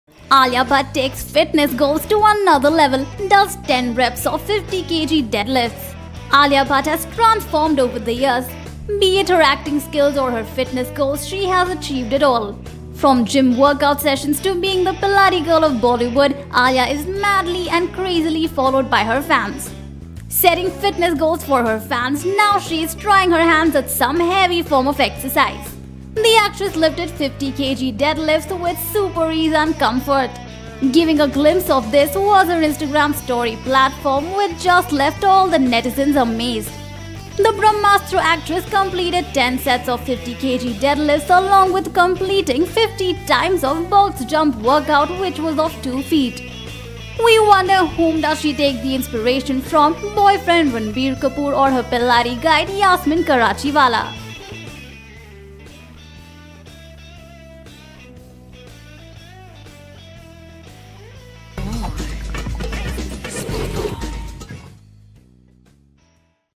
Alia Bhatt Takes Fitness Goals To Another Level, Does 10 Reps Of 50 Kg Deadlifts | SpotboyE
Actress AliaBhatt gives us fitness goals with yet another workout session. Lifts 10 sets of 50 kg deadlifts and completes 50 reps of box jumps.